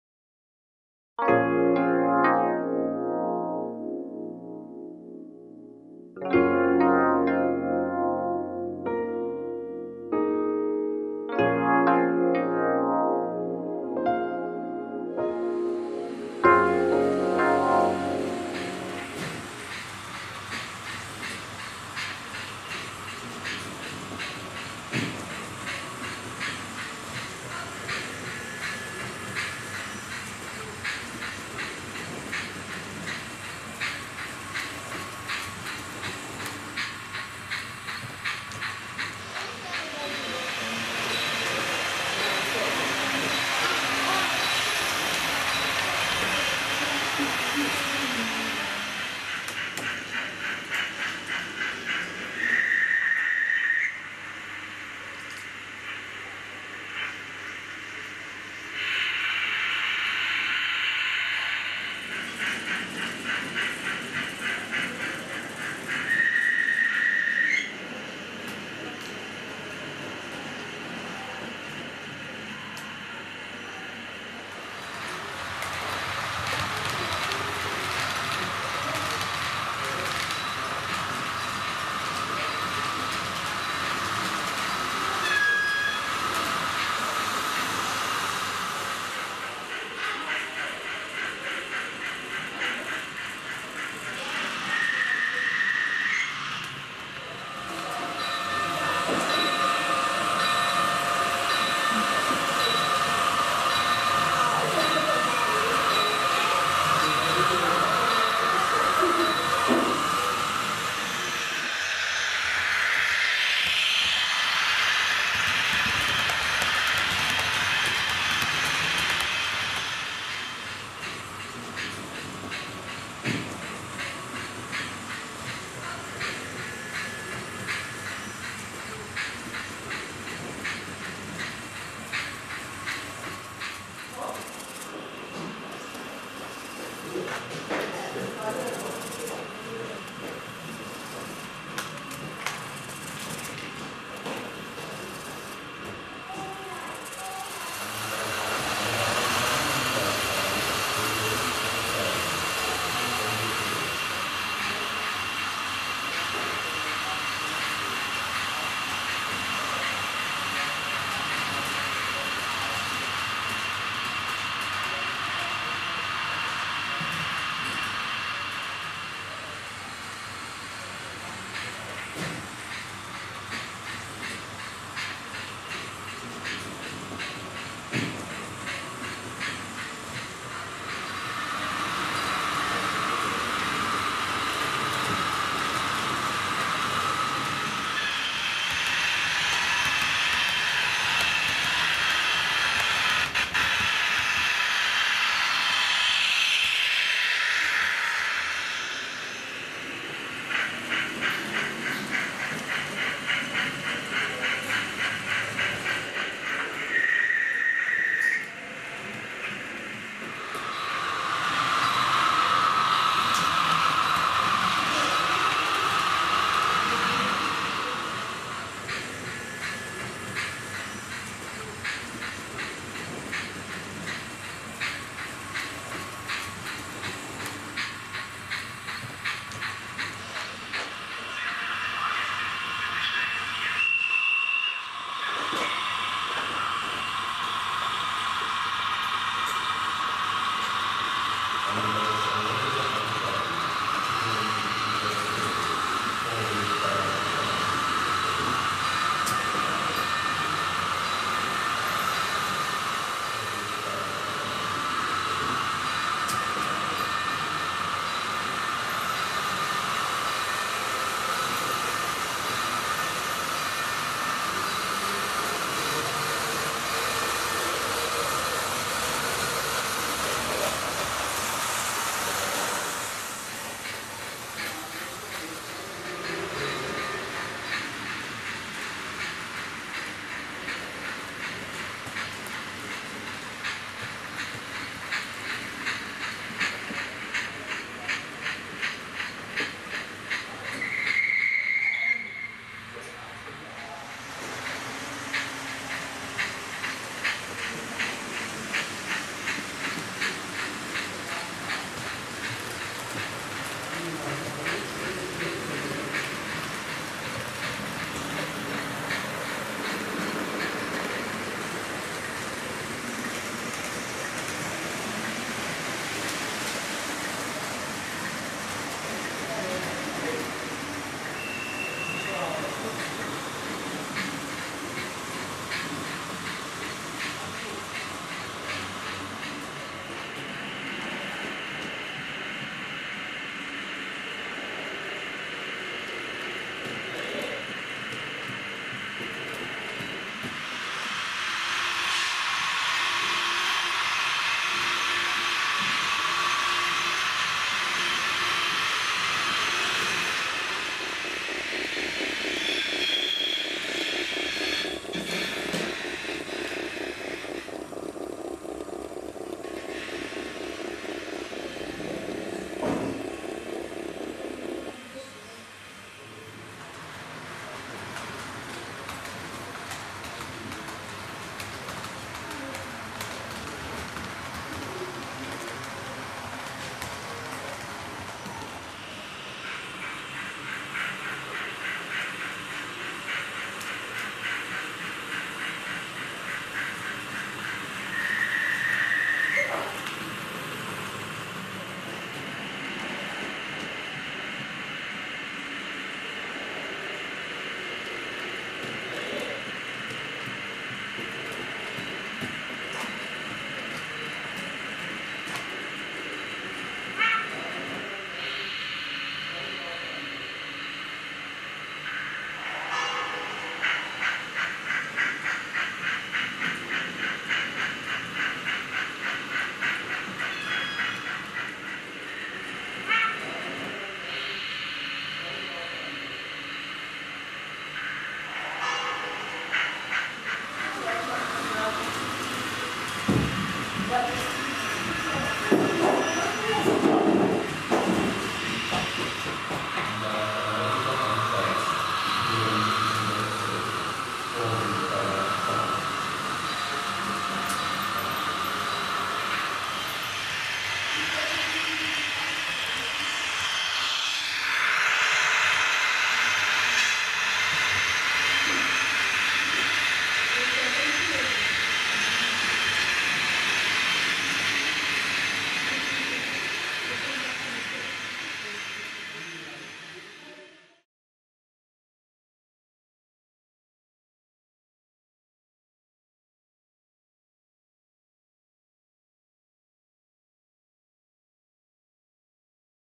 Rail Transport Modeling in Germany: A small model train exhibition in HO scale
This model train video is about a small permanent model railway layout near Frankfurt, Germany. That exhibition is called “Modellbahnhof Stockheim”, and it was constructed in HO scale. It is housed in a former railway station building. Except for the model trains, the exhibition is also known as a model railway museum preserving the regional railway history. On the layout, there are famous German steam locomotives, diesel multiple units and railcars used by today’s German railway companies. The layout, especially the model landscaping, is a bit older. Nevertheless, the trains are controlled automatically via DCC.